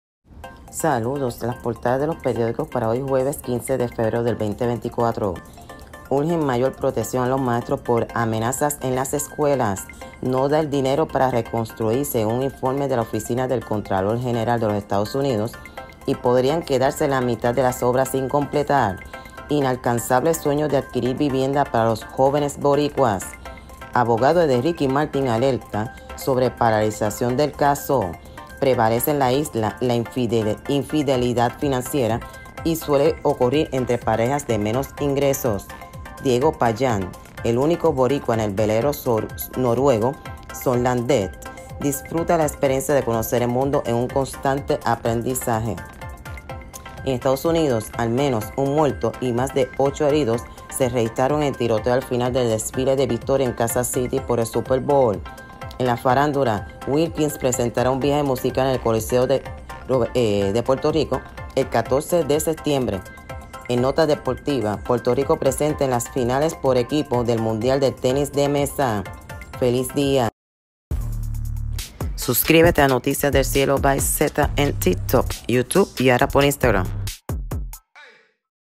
PORTADAS PERIÓDICOS JUEVES 15/FEBRERO/2024
Las portadas de los periódicos para hoy jueves 15 de febrero de 2024.

Noticias Locales
Urgen mayor protección a los maestros por amenazas en las escuelas. No da el dinero para reconstruir según informe de la Oficina del Contralor General de los Estados Unidos y, podrían quedarse la mitad de las obras sin completar. Inalcanzable sueño de adquirir vivienda para los jóvenes boricuas. Abogado de Ricky Martin alerta sobre paralización del caso. Prevalece en la Isla la infidelidad financiera y suele ocurrir entre parejas de menos ingresos. Diego Payán el único boricua en el velero noruego Sorlandet disfruta la experiencia de conocer el mundo en un constante aprendizaje.

Noticias Internacionales
Al menos un muerto y más de ocho heridos se registraron en tiroteo al final del desfile de victoria en Kansas City por el Super Bowl.

Noticias de Farándula
Wilkins presentará un viaje musical en el Coliseo de Puerto Rico el 14 de septiembre.

Notas Deportivas
Puerto Rico presente en las finales por equipo del mundial del tenis de mesa.

Reseña de Zulma Rodríguez para NoticiasDelCieloByZ (2024)
Feliz y bendecido día